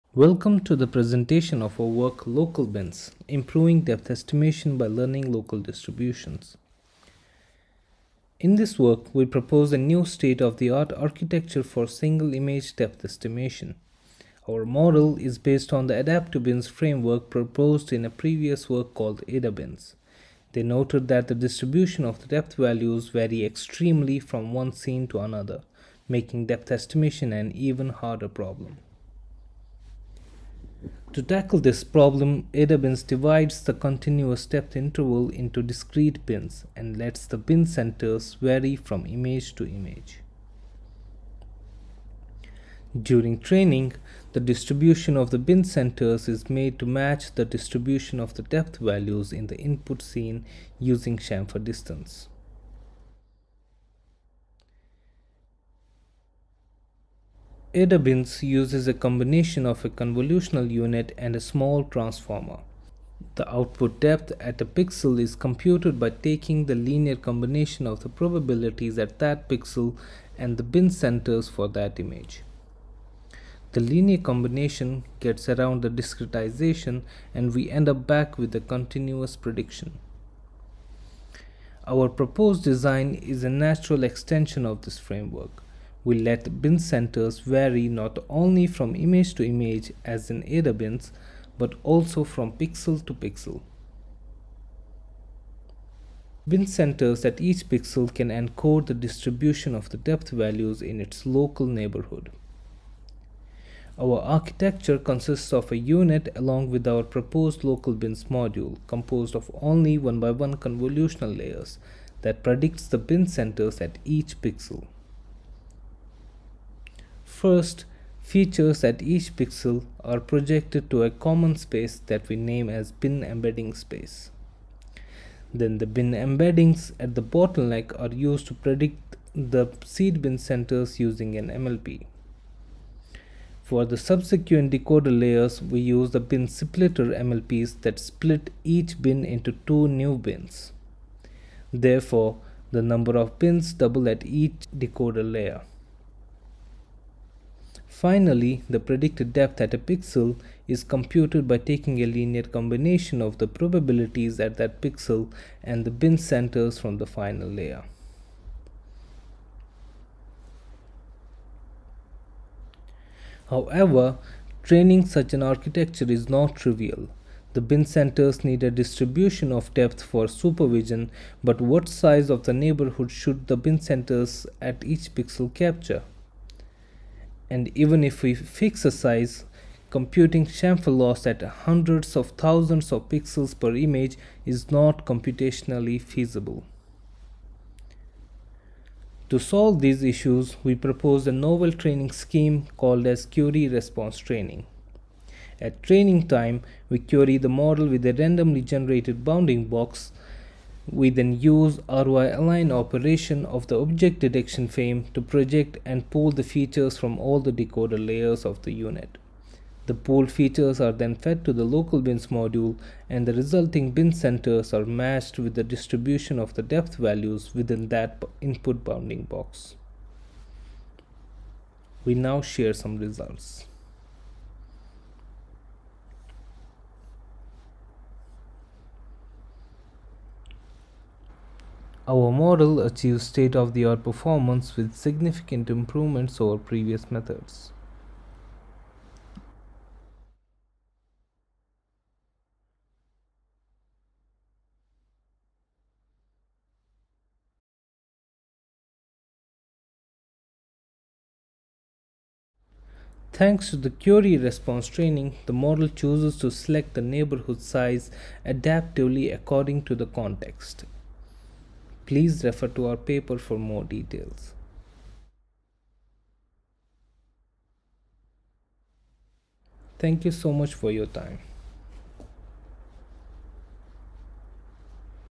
LocalBins: Improving Depth Estimation by Learning Local Distributions
SF Bhat, I Alhashim, P Wonka
ECCV 2022

Short Paper Presentation